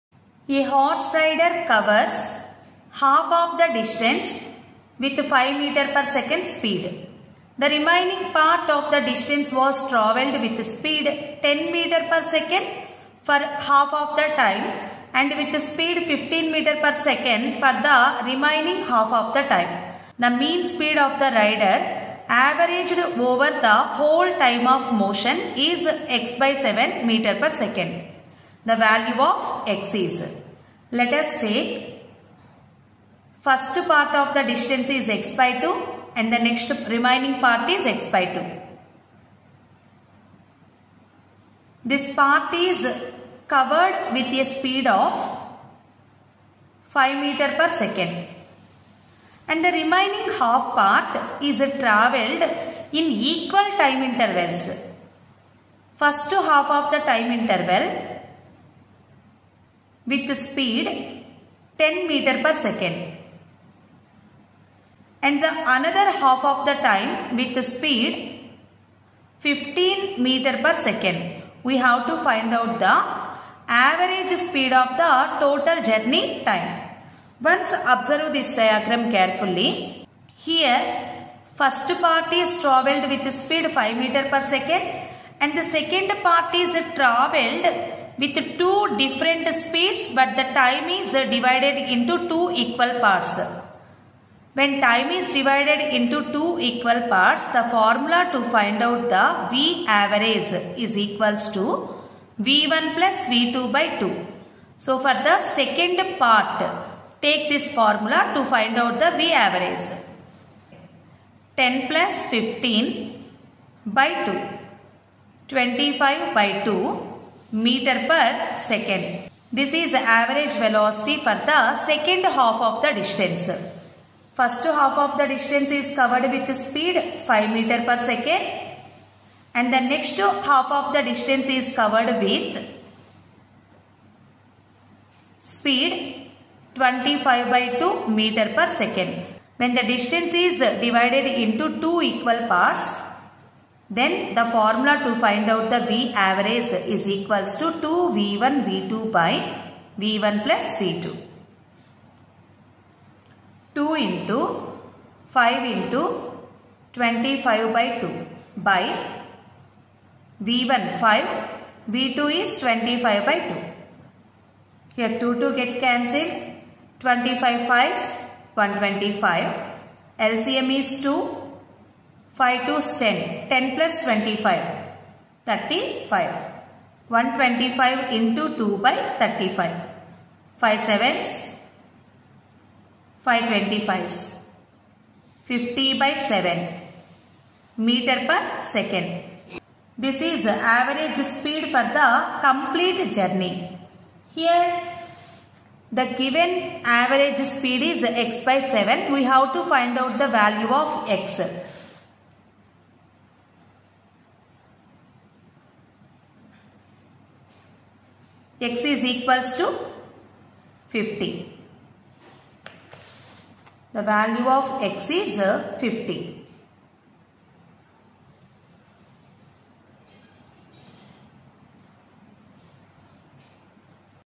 A horse rider covers half of the distance with 5m/s speed.The remaining part of the distance was
A horse rider covers half of the distance with 5m/s speed.The remaining part of the distance was travelled with speed 10m/s for half of the time and with speed 15m/s for other half of the time.The mean speed of the rider averaged over the whole time of motion is x/7 m/s.The value of x is
JEE MAINS 2023 QUESTION FROM KINEMATICS